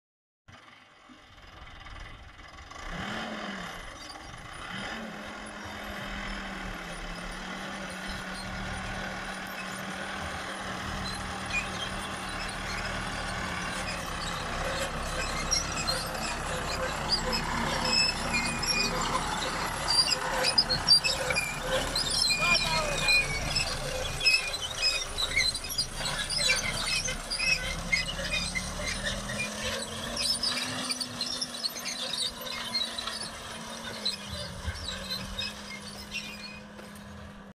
PLANTING MAIZE USING A TRACTOR.
This planter is mounted on the tractor, It plants 6 line at ago. The people behind is to ensure that each driving wheel is moving coz it easy to get stuck. It works batter in dry time.